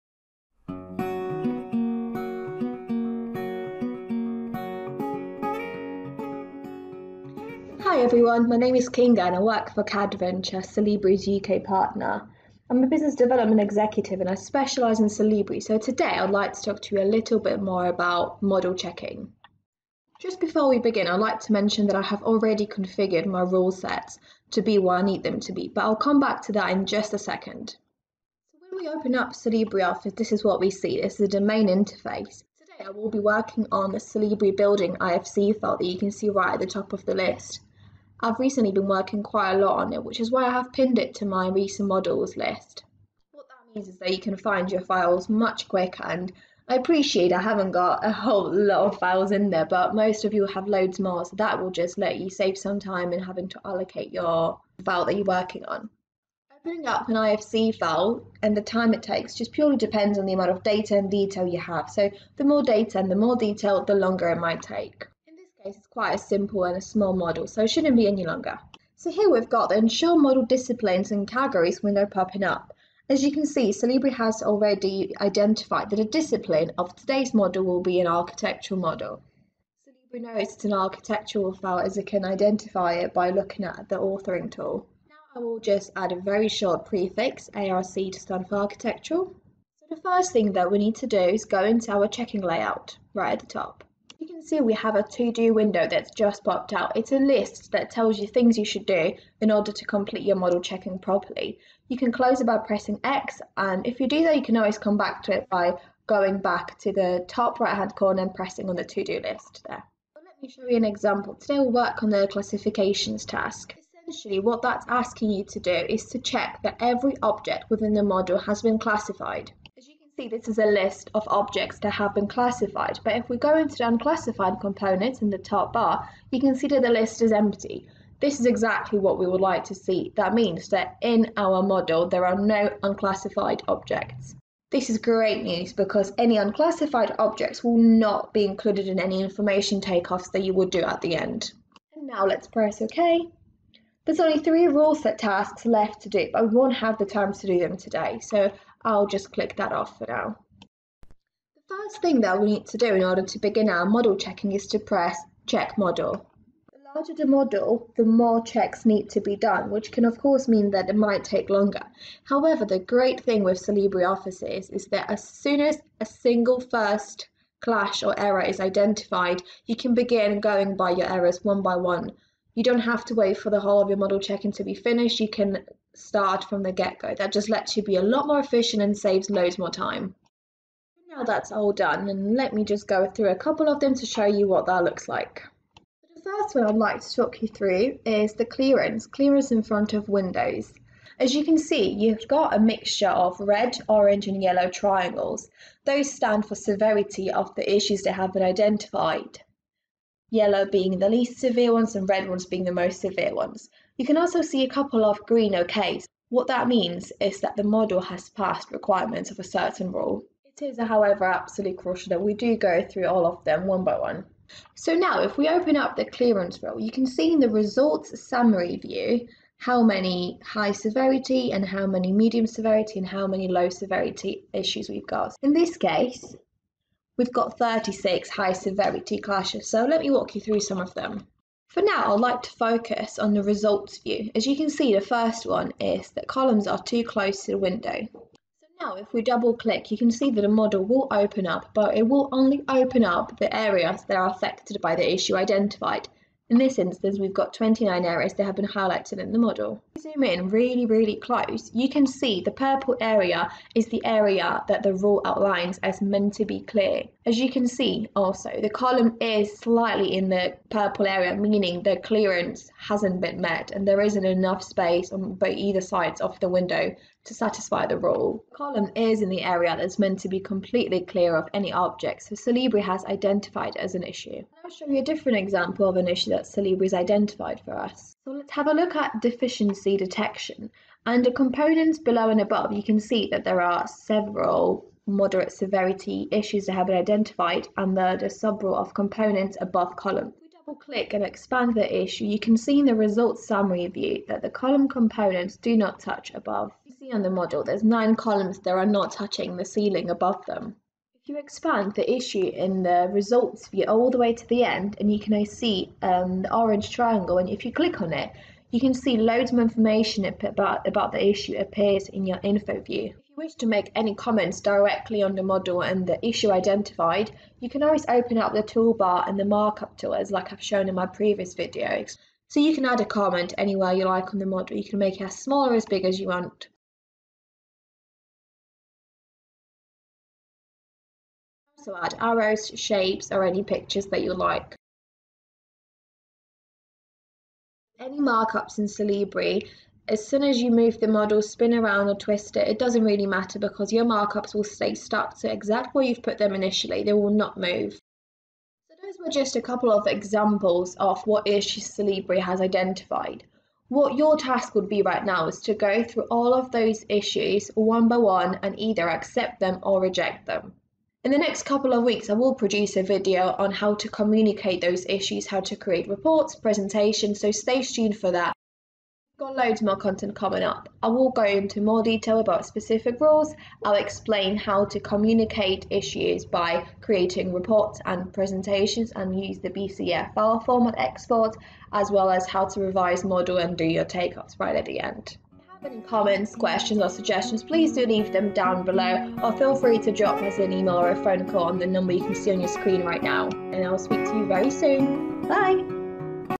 Solibri Office - Model Checking
Start your Solibri journey with our Cadventure tutorial.

Solibri Office is for model checking -  and so much more. With its unparalleled quality checking and flexibility, Solibri is loved by BIM professionals around the world.

Solibri takes your model quality to a level way beyond simple clash detection.